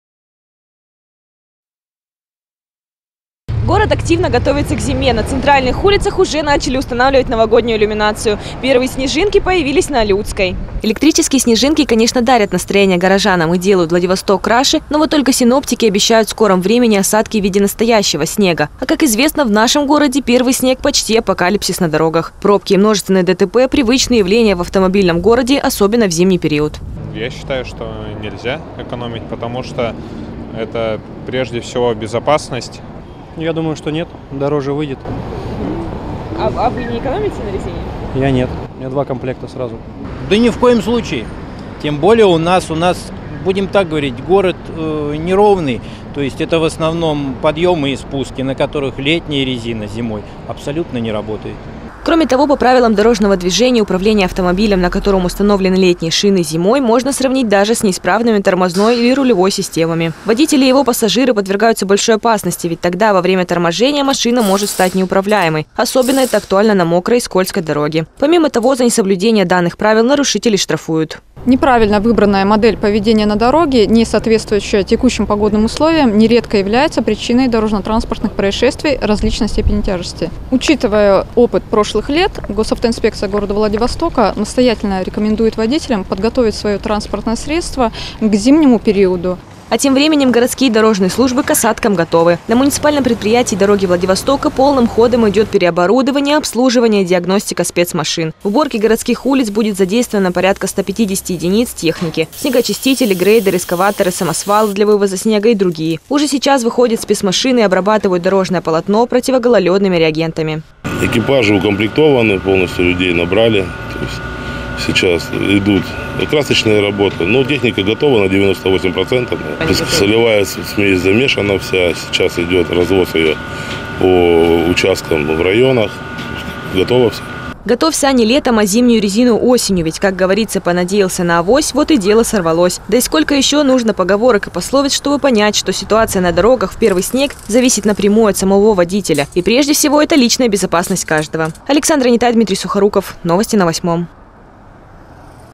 Городские службы готовятся к первым снегопадам
Несмотря на теплую осень, городские службы ведут тщательную подготовку к снегопадам. По сообщениям синоптиков, первые осадки в виде снега пройдут в крае уже в конце этой недели.